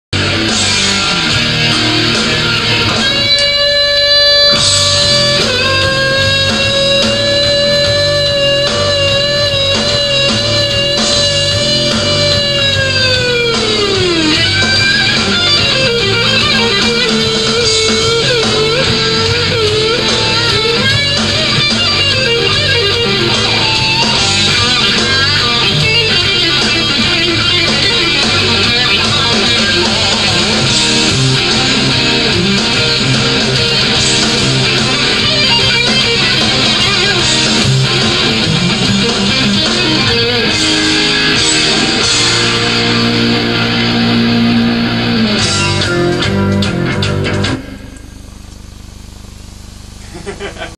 Paul gilbert in studio sings into pickups and then... laughts
Gilbert doing. what??? Can you answer to me?
EDIT: NOW WE KNOW! He just sings in the pickups, activating a sustainer or another distortion pedal before doing it. With his voice he can make drive bombs or a tremolo effect! At the last scream we can actually ear his voice singing "uh!"